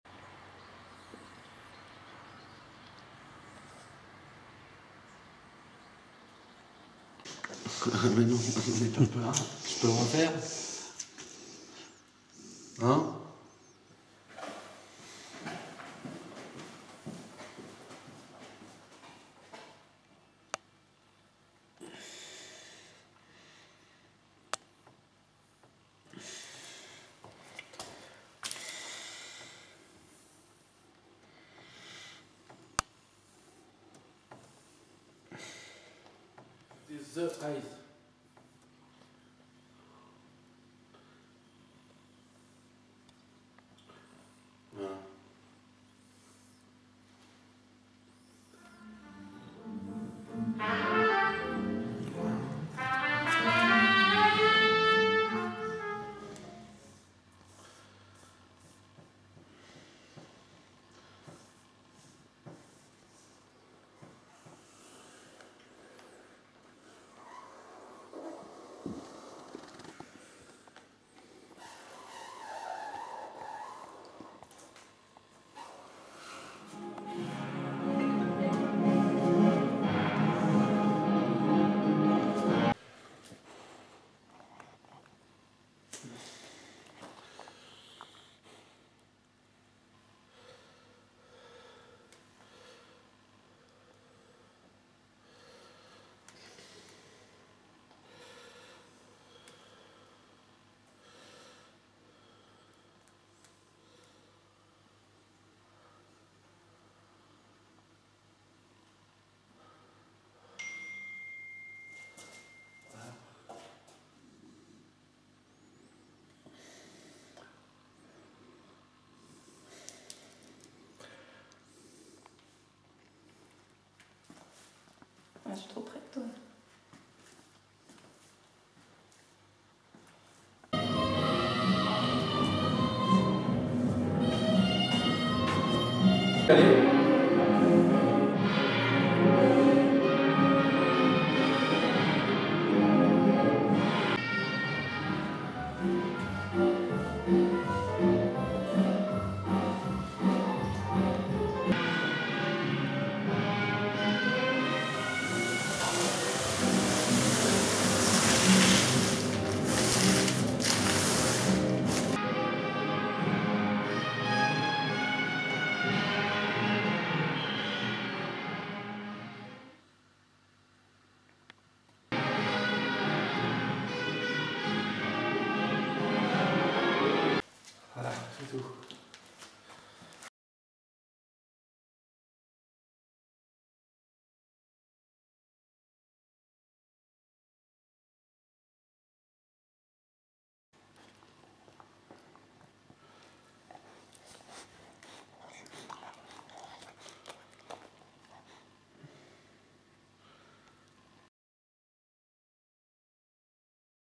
Summer guest: Philippe Bonan painting on portraitrs of artists we like